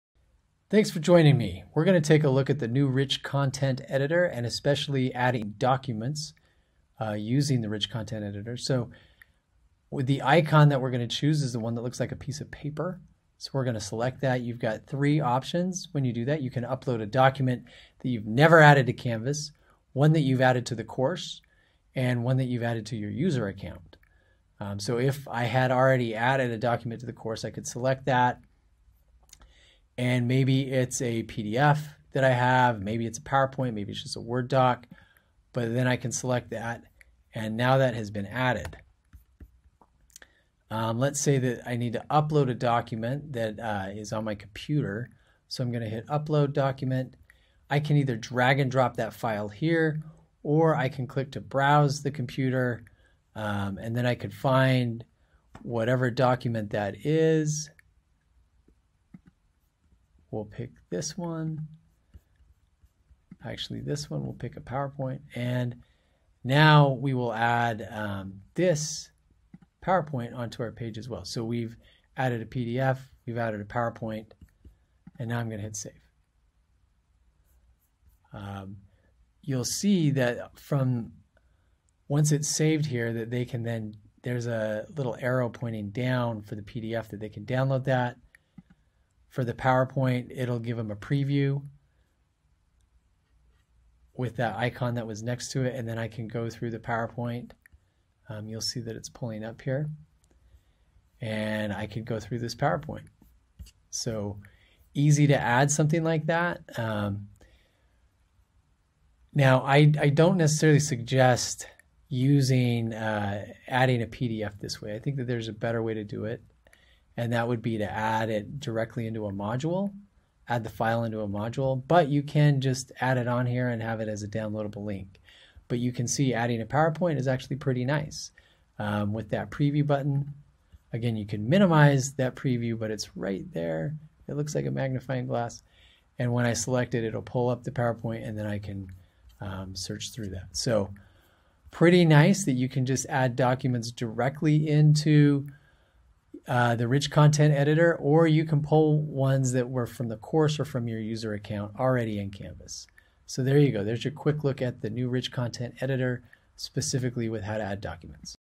Adding Documents in RCE
Join Justin Fawson from our Professional Development team on this tutorial about the Rich Content Editor in Canvas. In this video we show how to add documents.